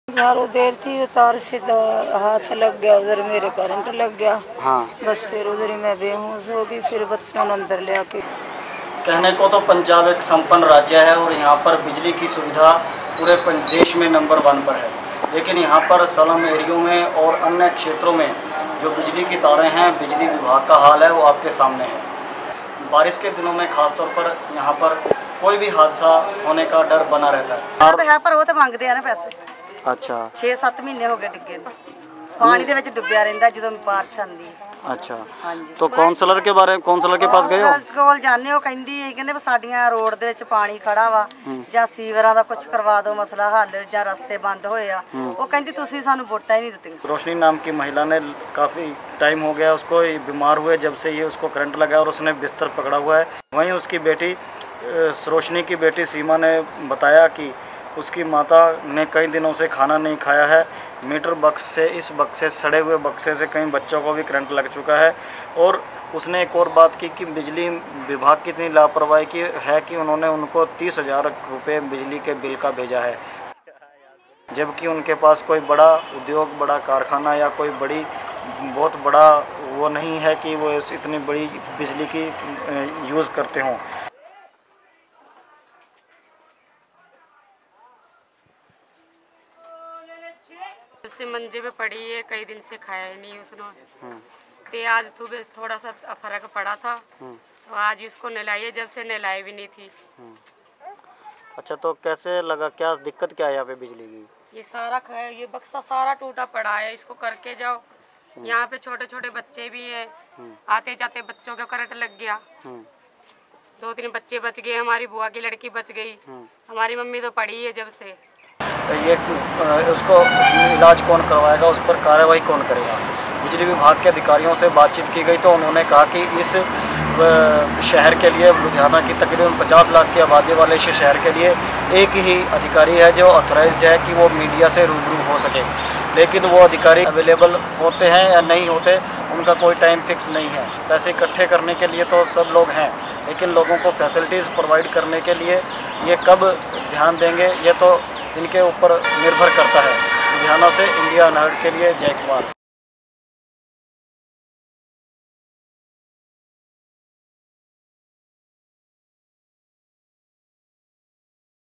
Live Wires of Ludhiana
People fear electrocution as monsoons submerge open wires.

The residents of Sanjay Gandhi Colony, Ludhiana, are in constant danger of being electrocuted. Wires and circuit boxes have lay bare and exposed to the monsoon rains since the past five years, posing a great threat to passersby.

In today?s video, Community Correspondent Jai Kumar exposes the terrible state of infrastructure in one of Ludhiana?s poorest areas. Sanjay Gandhi Colony comprises a thousand households, almost all of them earning an income below poverty line. A majority of the residents are labourers from Punjab and other states.

The Punjab State Electricity Board is in charge of supplying electricity to the colony. ?As per the law, BPL households are entitled to 200 units of free electricity every month. No one here gets free electricity. Can?t the board ensure the residents? basic safety at least?? asks Jai Kumar.

This monsoon, Rutvi Devi, a widow and the breadwinner of her family, got electrocuted while sweeping the ground. She has been bedridden since then, and does not foresee her health getting any better in the near future. Jai Kumar tells us that many of Rutvi?s neighbours have children who play in the streets during the day. ?They are so worried about them that during the rains they refuse to let them play outside their houses.?

When asked if Rutvi?s family got any compensation from the government, he replied, ?The family hasn?t bothered asking for compensation. They are poor and do not have a voice against authority. They don?t expect the government to listen to them.?

?While the prosperous parts of Ludhiana get constant and safe supply of electricity at all times of the day,? says Jai Kumar, ?the poor are refused even basic infrastructure. They are asked to pay for electricity that they have not received, and pay for damages that they have not caused. Where can they get the money for this? It?s not possible.?

-Rajyashri Goody